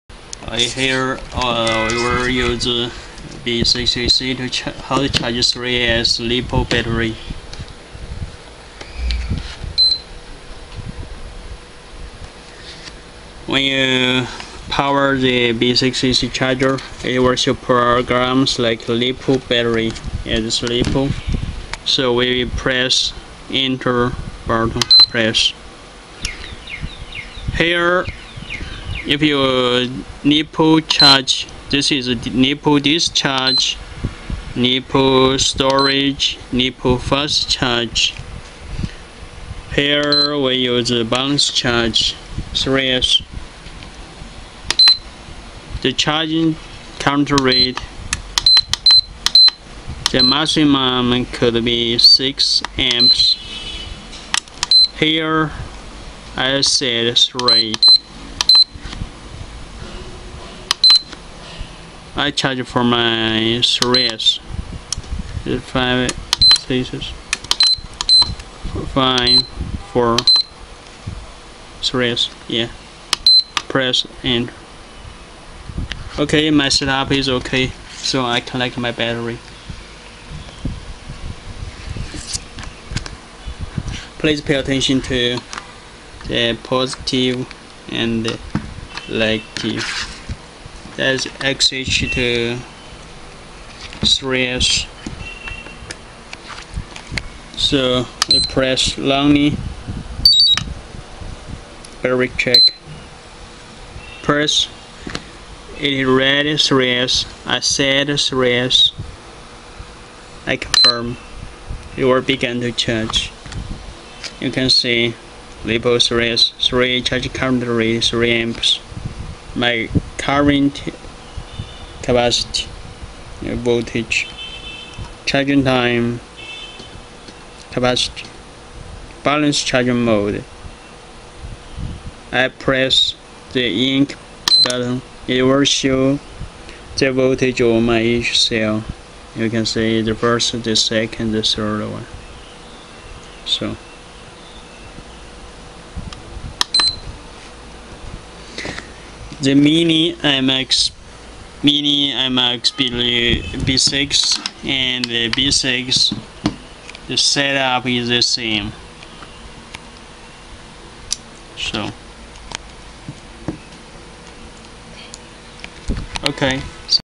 IMAX B6AC Balance charger to charge 3S Lipo
1.Power up the charger
2.Choose LipoBatt Press Start
3.Choose charging type,like Blanace,Fast,Descharge,etc.
4.Set charging volts,like 2S,3S,4S .. and Charing current,max. is 6Amps.
5.When set-up is ok,connect your batter port to the Charger,pay attention to the positive and negative.
5.Press the fourth button(Start) for 3 secons,then press again to confirm charge.
6.Press the third button will show the each cell volts of your battery.